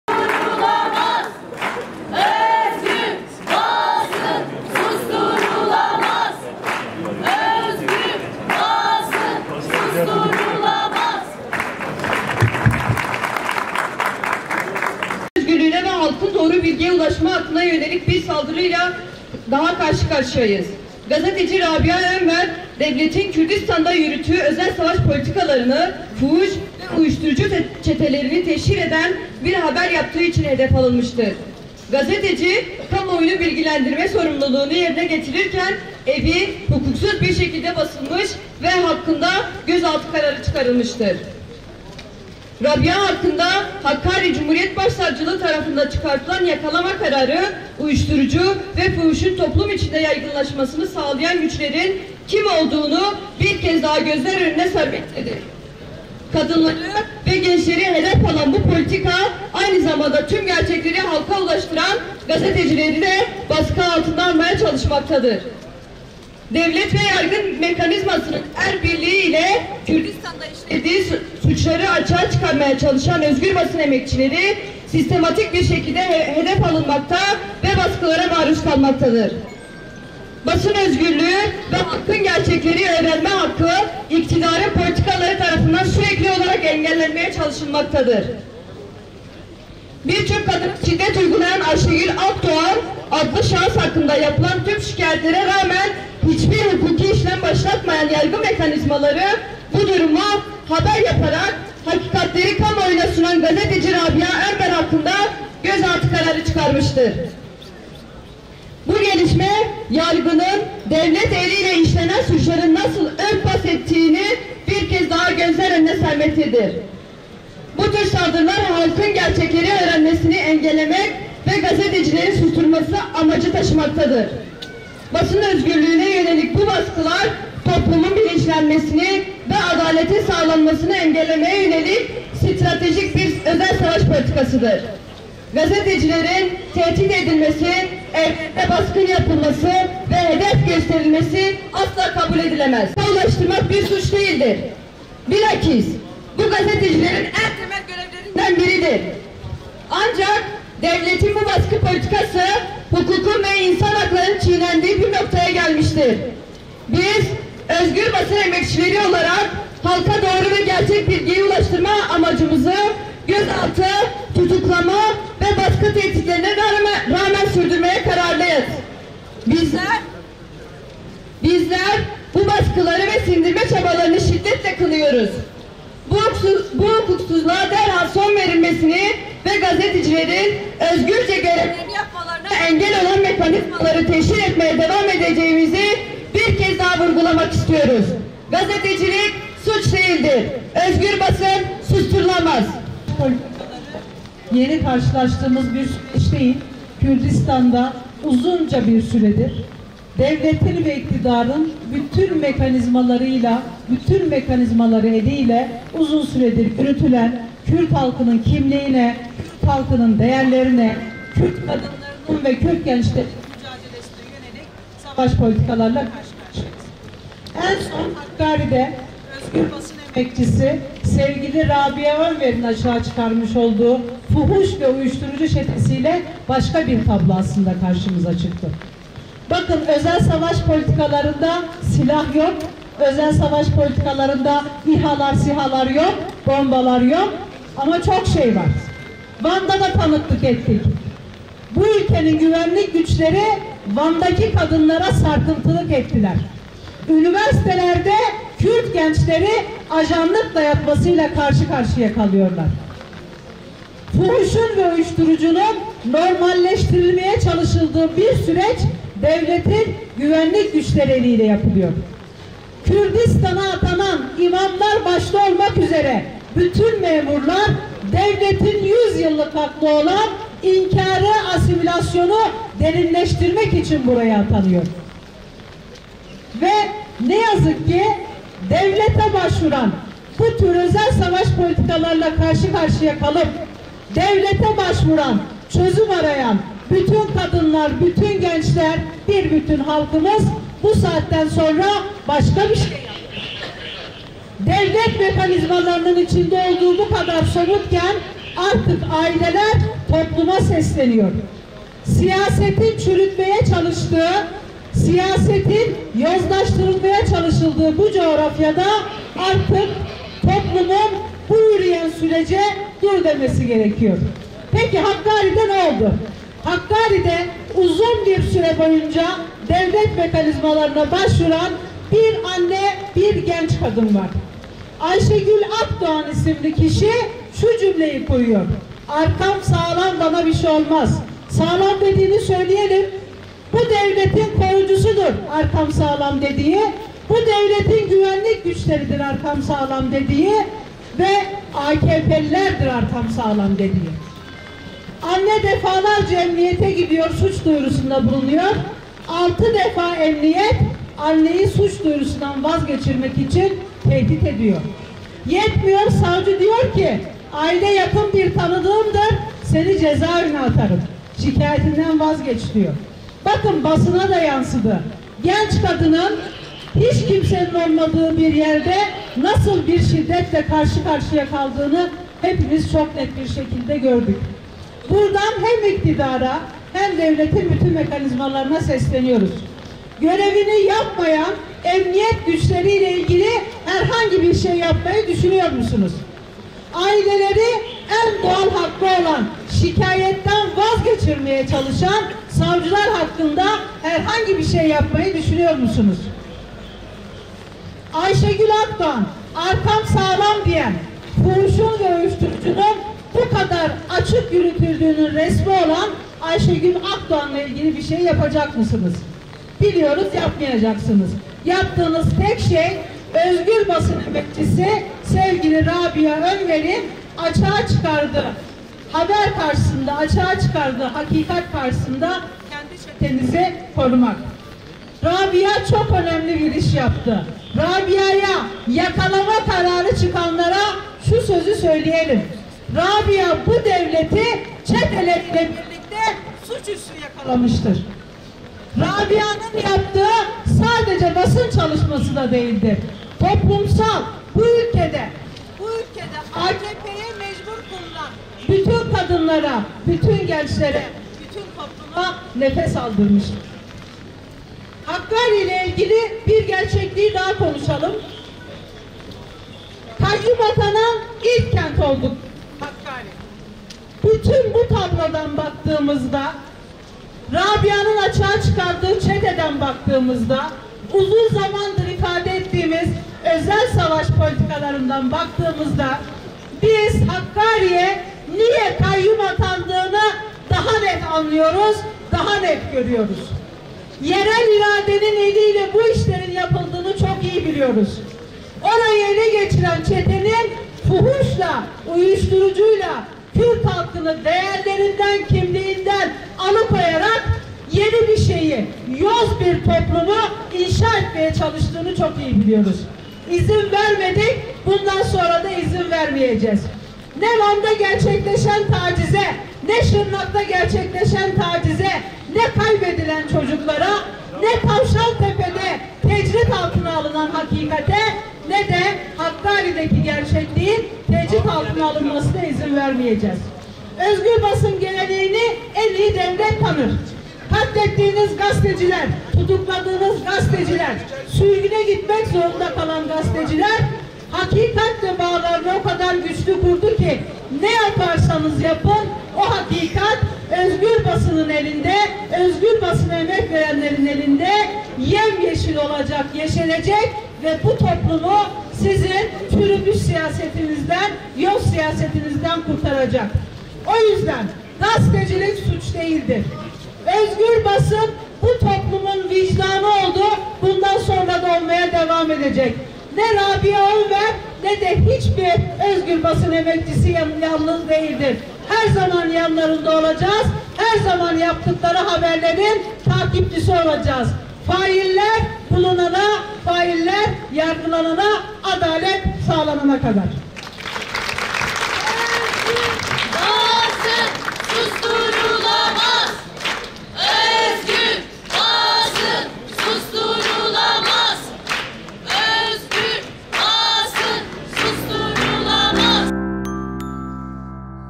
HAKİKATİN TECRİT ALTINA ALINMASINA İZİN VERMEYECEĞİZ
Jinnews Muhabiri Rabia Önver için Wan’da yapılan açıklamada konuşan DBP Eş Genel Başkanı Çiğdem Kılıçgün Uçar, “Hakikatin tecrit altına alınmasına izin vermeyeceğiz. Ne yaparsanız yapın o hakikat özgür basınının elinde yeşerecek ve bu toplumu çürümüş siyasetinizden kurtaracak” dedi.

DAHA FAZLA BİLGİ İÇİN:
TECRİT ALTINA ALINMASINA İZİN VERMEYECEĞİZ